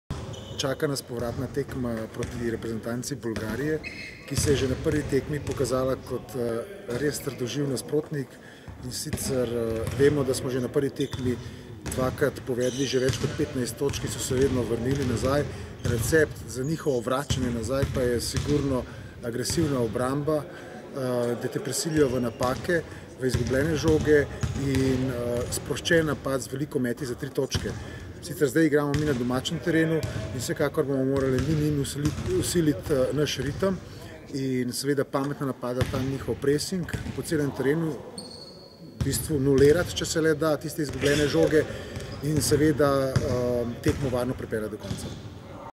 Rado Trifunović o reprezentanci Bolgarije
Moško člansko reprezentanco drevi v Stožicah čaka peta tekma kvalifikacij za nastop na evropskem prvenstvu. Nasprotnica bo Bolgarija, zmaga na tej tekmi pa bi slovensko vrsto popeljala na prag preboja na EuroBasket.